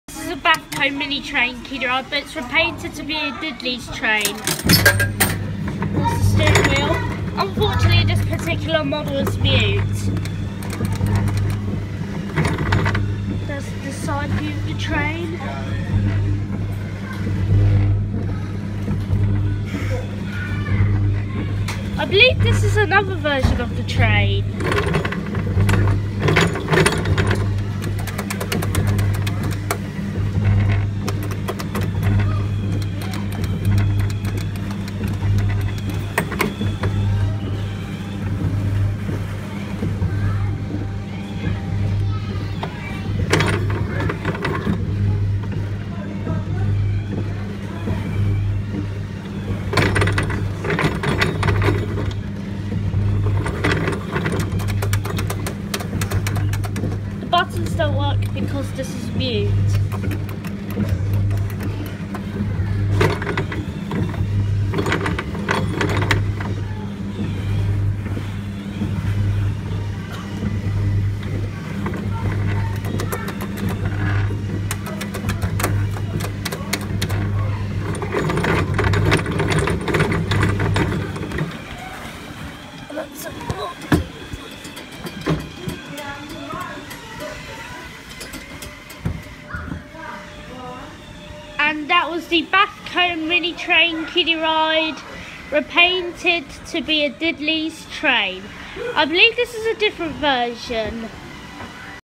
Bafco Mini Train Kiddie Ride (V2) (As A Diddlys Train)
Filmed At Brooklands Pleasure Park In Worthing On April 19th 2017

Made And Manufactured By Bafco In The 1990s. This Ride Was Supposed To Make Trains Sounds, But It Was Mute. And They Repainted This To Look Like A Diddlys Train, I Was Disappointed That They Repainted This Ride. This Ride Is Rare But It Was Popular Back In Its Day. I Believe This Is The Second Version